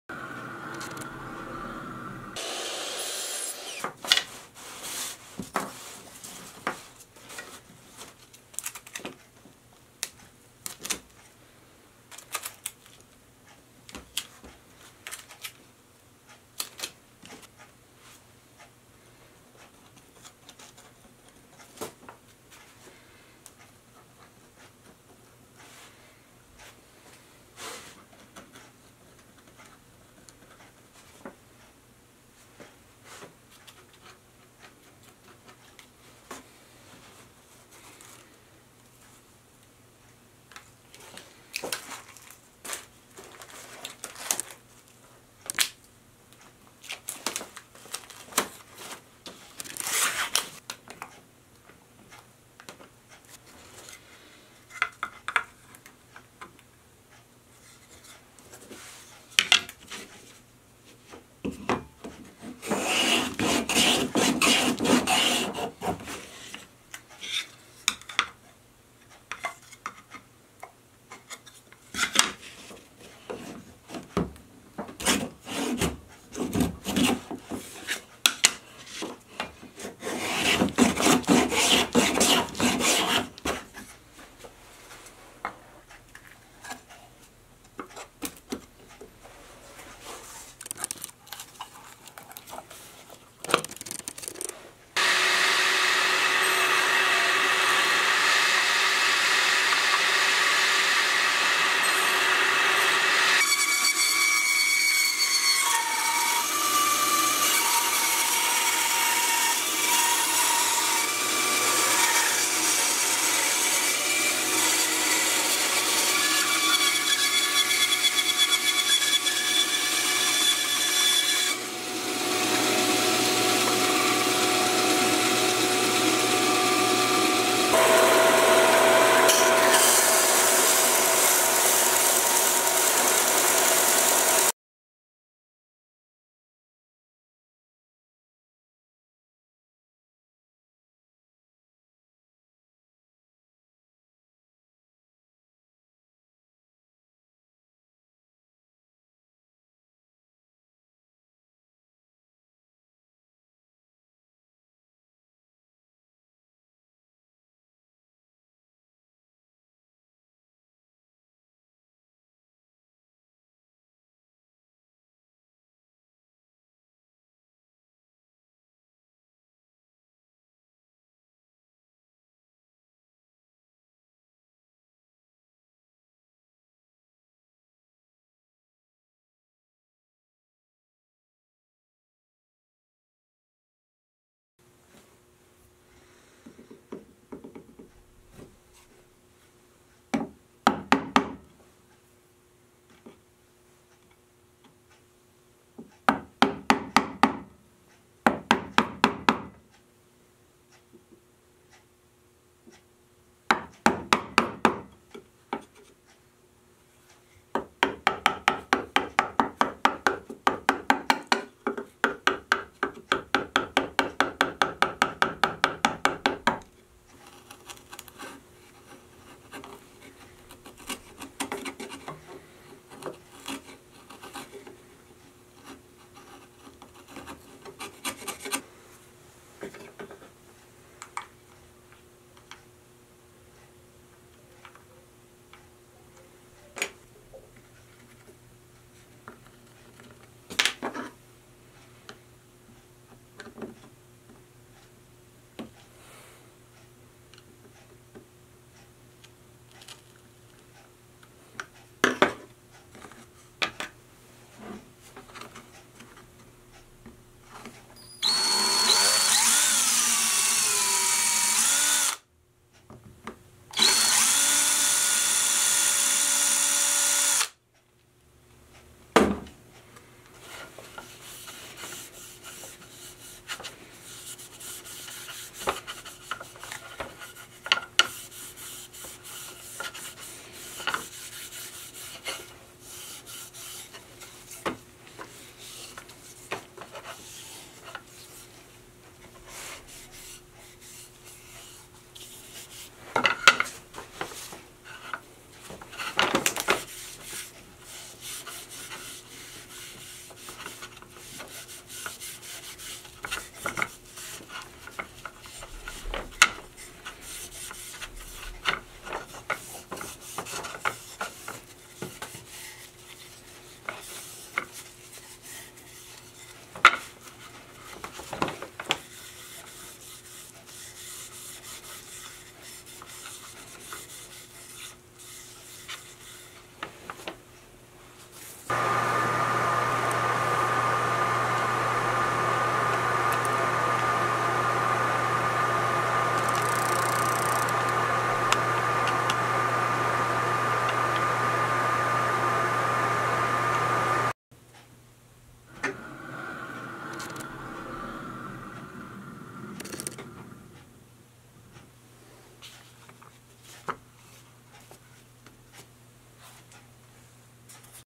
Walnut pencil case full build video
This video shows my building process for a walnut wood pencil case. This includes how I cut and assembled the sides and fit the faces of the box. Then cut the box into two halves on the bandsaw. Finally, finishing the wood with Danish Oil and attaching the hardware.